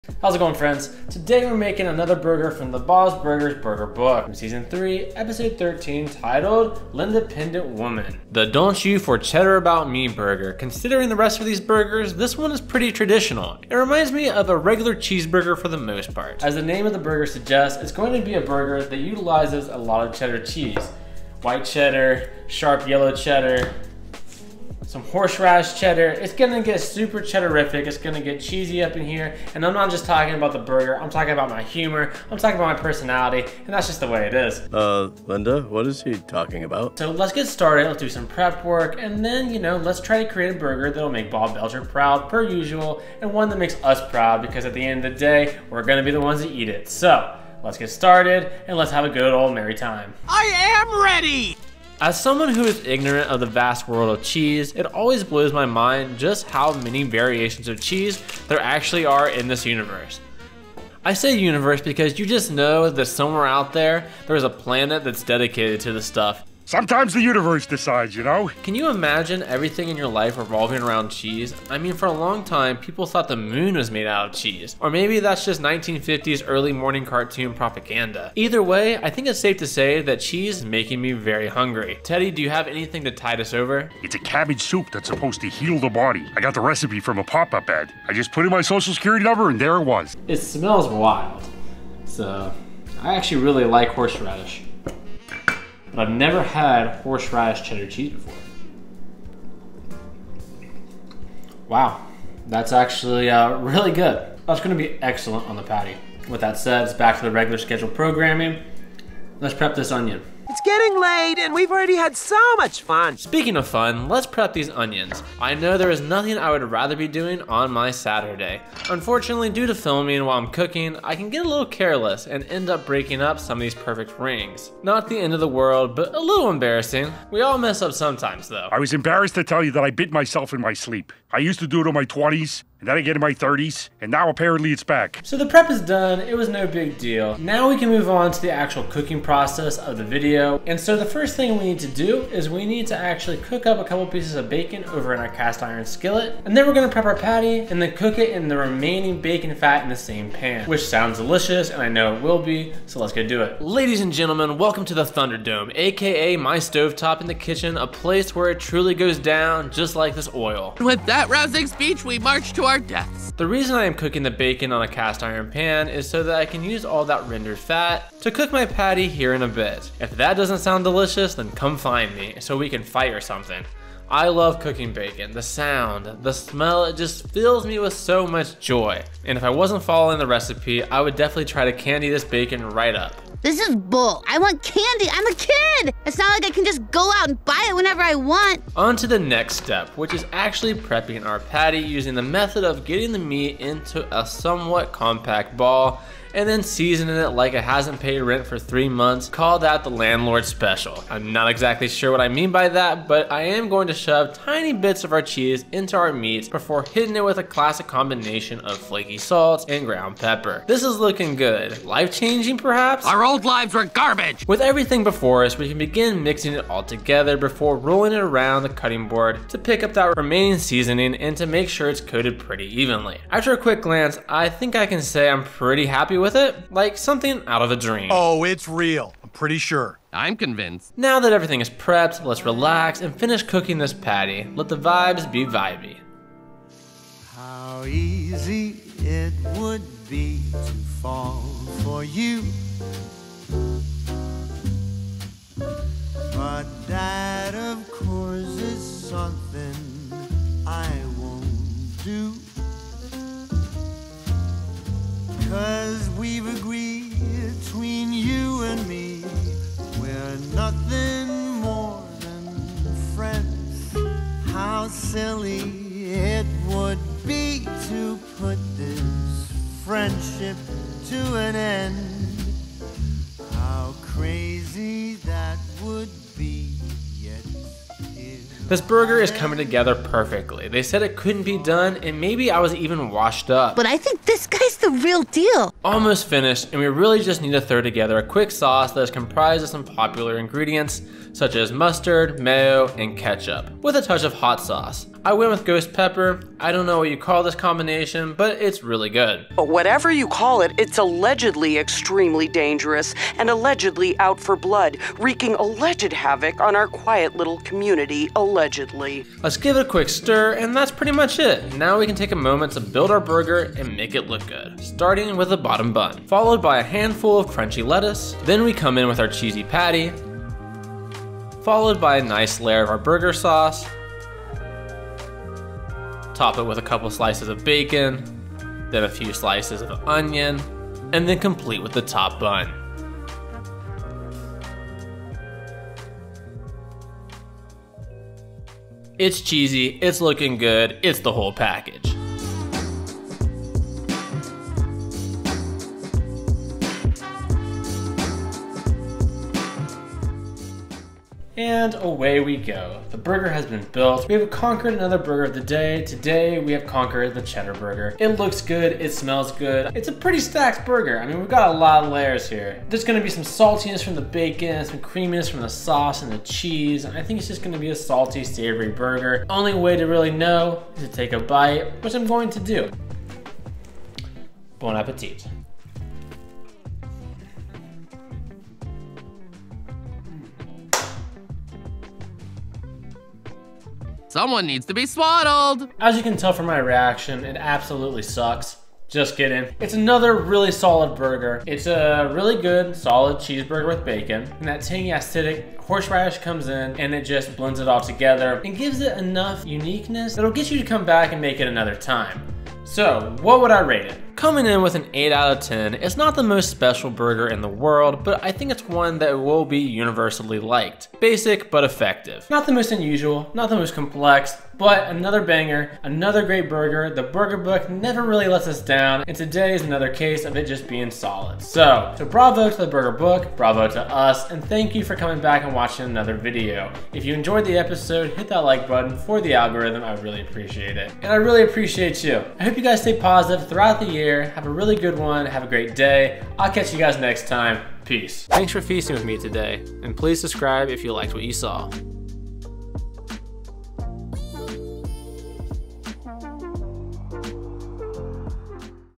Cooking EVERY Burger in Bob's Burgers Ep. 53 “Don't You Four Cheddar Bout Me”
Join me on my culinary journey to cook every single burger from one of the best animated shows on the planet, Bob's Burgers. We will start with cooking the first 1 through 72 listed in the Burger Book and then go from there. Today's burger is  the “Don't You Four Cheddar Bout Me”.

Enjoy my hungry friends.

Ingredients:

Ground Beef
Buns
Salt
Ground Pepper
Bacon
White Sharp Cheddar
Yellow Sharp Cheddar
Horseradish Cheddar
Lettuce
White Onion
Yellow Mild Cheddar

Feast on Film Links